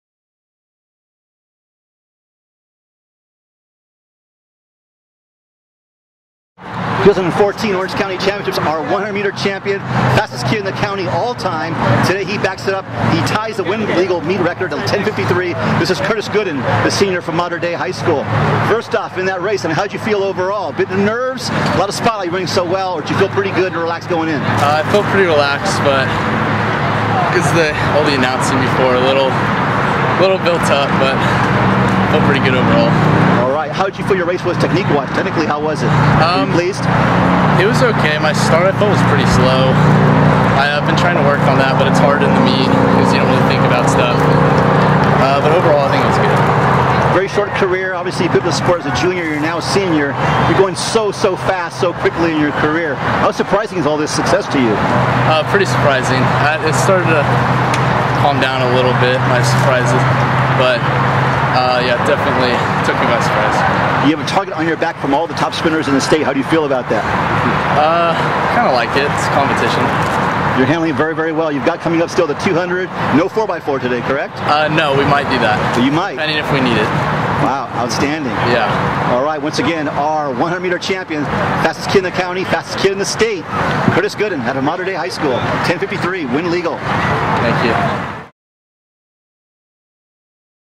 Curtis Godin - Mater Dei @ 2014 Orange County Track & Field Championships
Interview with Mater Dei HS senior Curtis Godin after he tied the meet record for 100 meters at the 2014 Orange County Championships of Track & Field on April 26th @ Mission Viejo HS. Curtis ran a wind legal 10.53